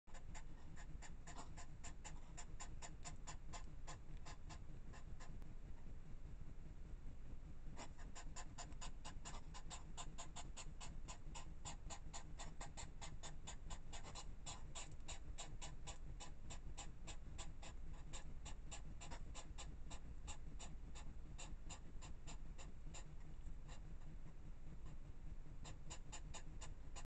He Might Be Smaller But He Is The Boss
The smaller Hedgehog shows the bigger Hedgehog who is the boss at Feeding Station 1